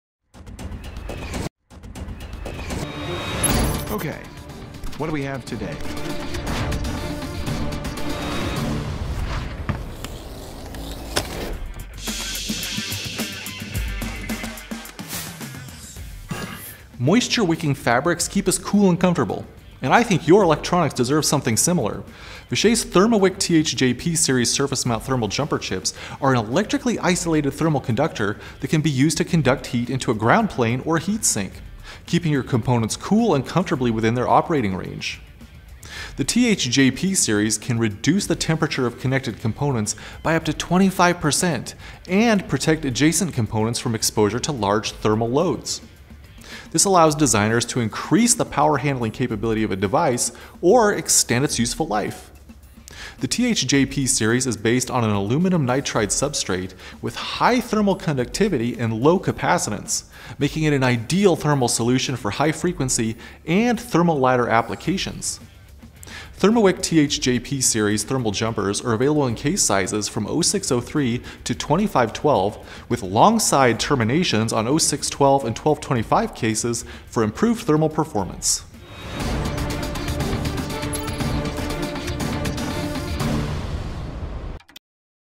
Vishay ThermaWick Thermal Jumper Surface Mount Chip | Tech Specs
Vishay’s ThermaWick™ THJP series surface mount thermal jumper chips are an electrically isolated thermal conductor that can be used to conduct heat into a ground plane or heat sink, keeping your components cool and comfortably within their operating range.

Check out TTI, INC's profile on All About Circuits and see more technical articles they created